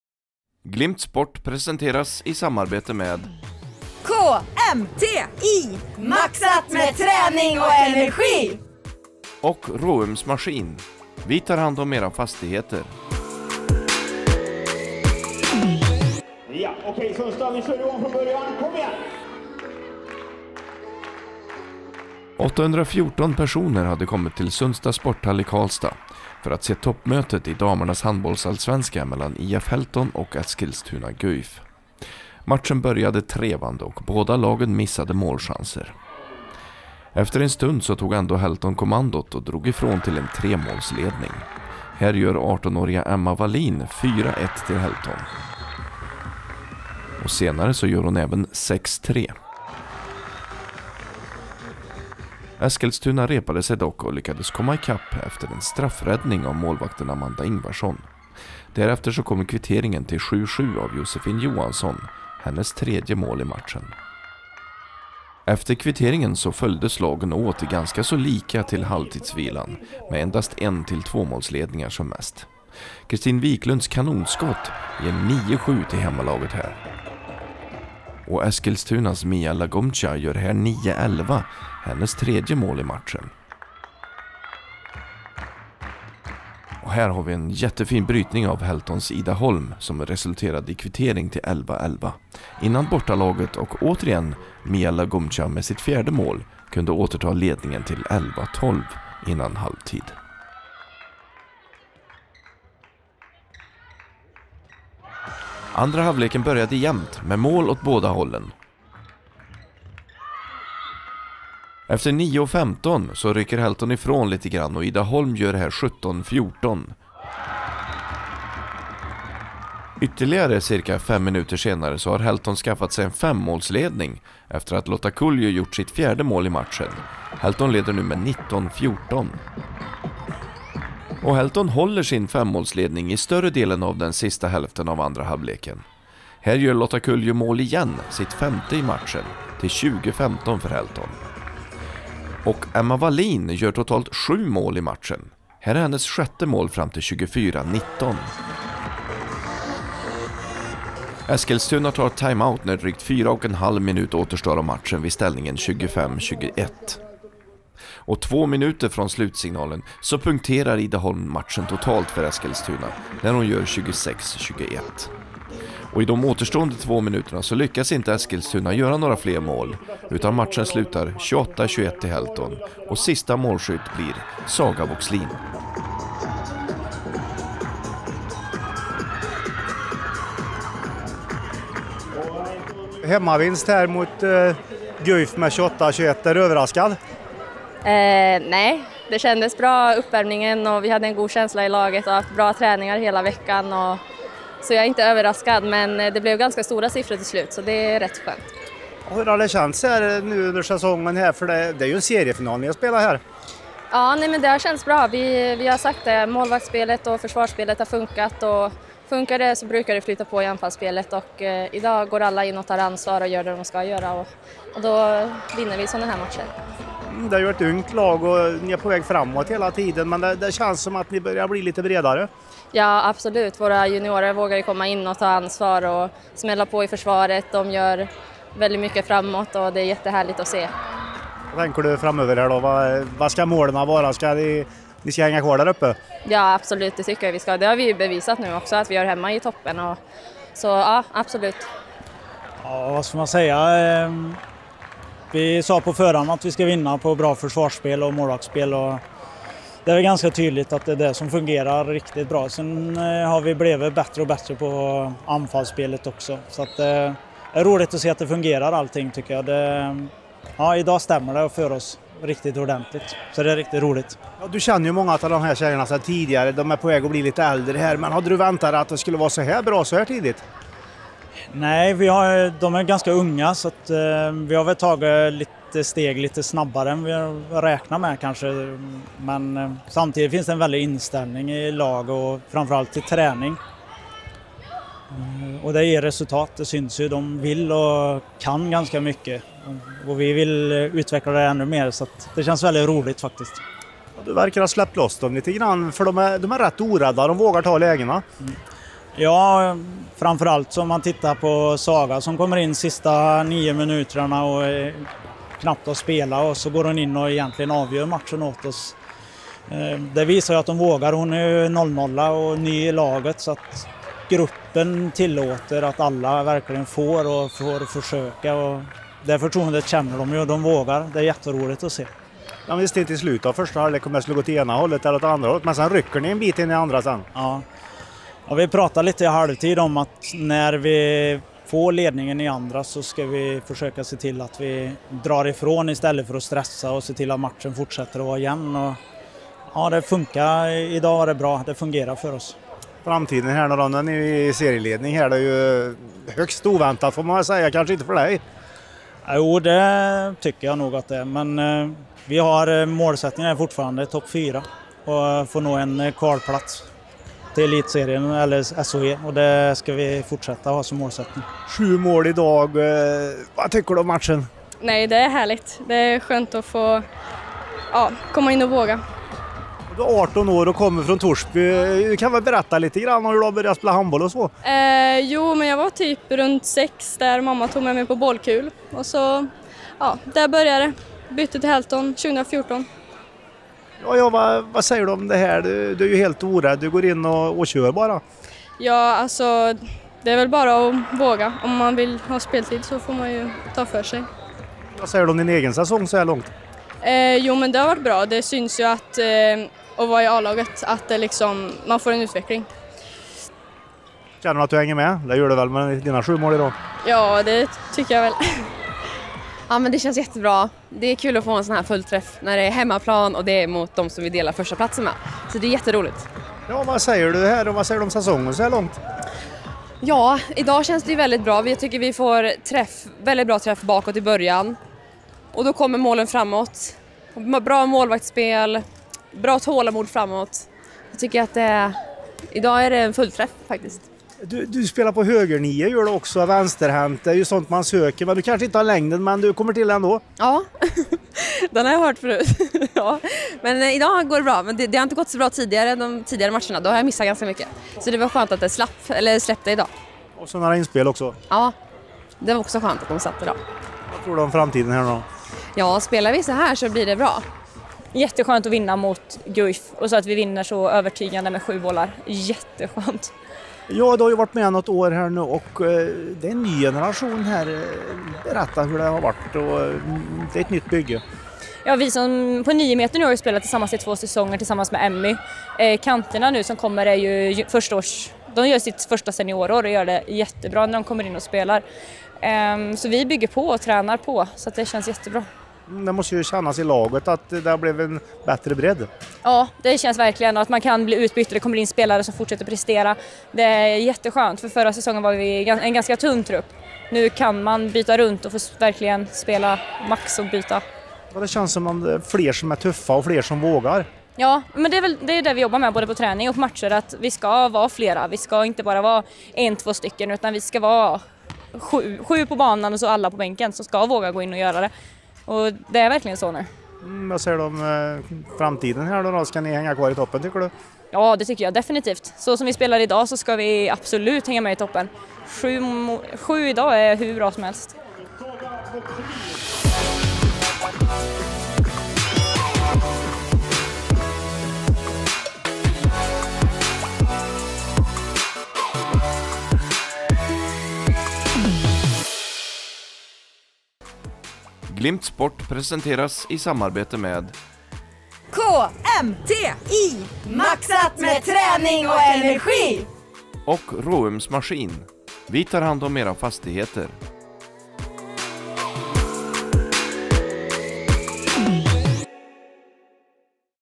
Toppmötet IF Hellton-Eskilstuna GUIF
Det var de två topplagen i damernas handbollsallsvenska som möttes i förra veckan. 814 personer hade kommit till Sundsta sporthall i Karlstad för att se den tidiga seriefinalen. Ni får här ett sammandrag med höjdpunkter från matchen samt intervjuer med spelare och ledare efter matchen.